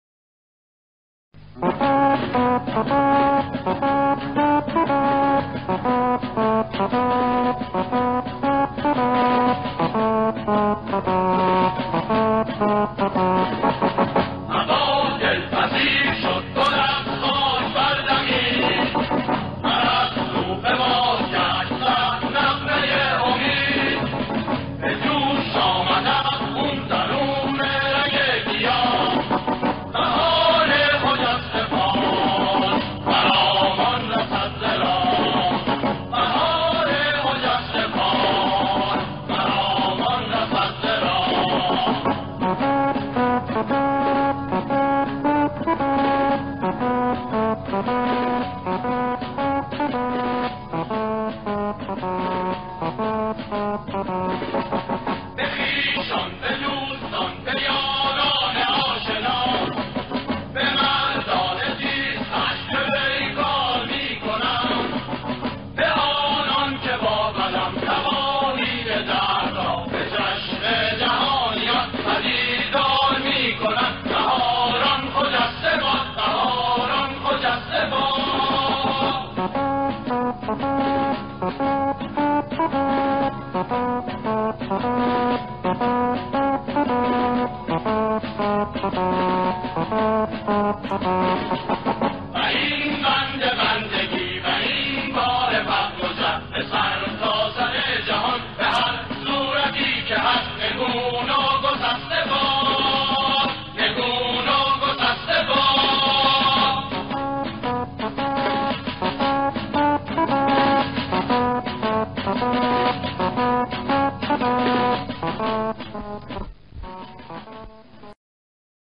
آزادی زندانیان سیاسی به دست مردم
فیلم کمتر دیده شده از آزادی آخرین دسته از زندانیان سیاسی زندان قصر از دست دیکتاتوری شاه
در ۲۱ دی ۱۳۵۷